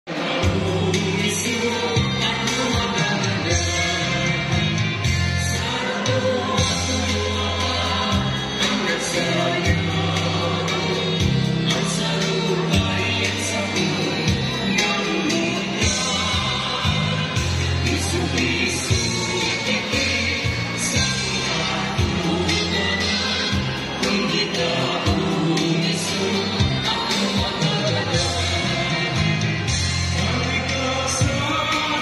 PAGTARABANG: 10th Year Anniversary and Christmas Party (Pantomina)
Bicol Traditional Dance
Taken during the PAGTARABANG 10th Year Anniversary and Christmas Party